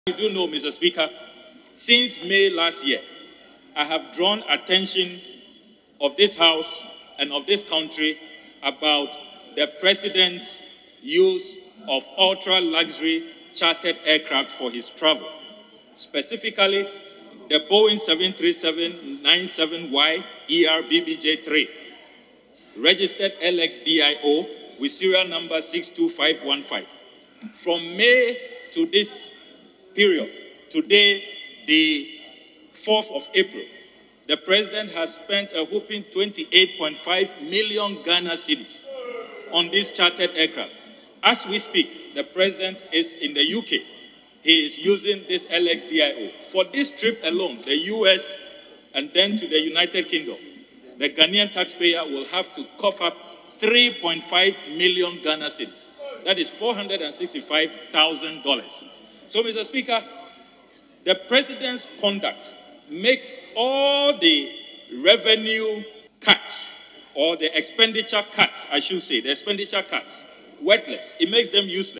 Samuel Okudzeto Ablakwa, takes on Akufo-Addo for his continuous use of a luxurious chartered flight
Member of Parliament for North Tongu, Samuel Okudzeto Ablakwa, takes on President Akufo-Addo for his continuous use of a luxurious chartered flight for foreign travels.